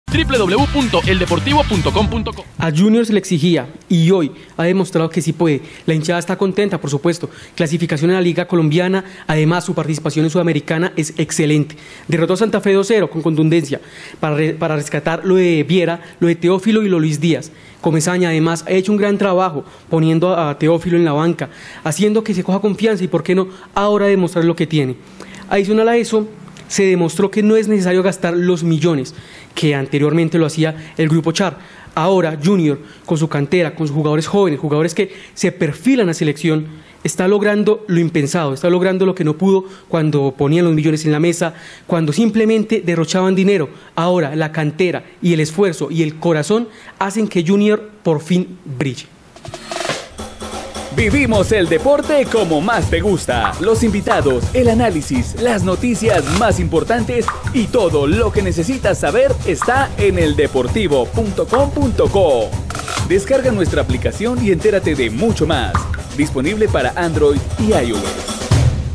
Modificaciones positivas en el Junior de Barranquilla
El conjunto 'Tiburón' reformó sus planteamientos y ahora da una lección a los demás equipos del FPC.

Videocolumna de Kevin Montien
Producción: Willam Fernando Cortés